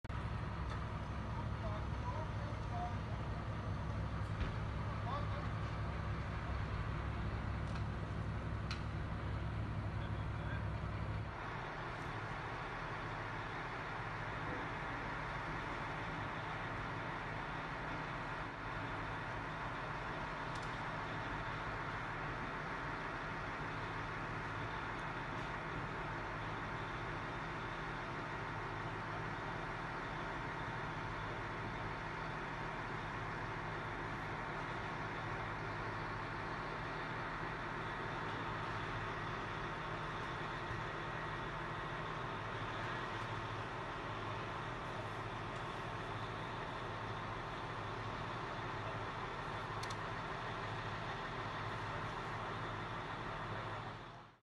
Versunkene Autos aus Saale in Halle gezogen
Halle (Saale), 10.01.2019: An der Saalepromenade unterhalb der Burg Giebichenstein in Halle sind am Donnerstag zwei Fahrzeuge aus dem eisigen Wasser geborgen worden.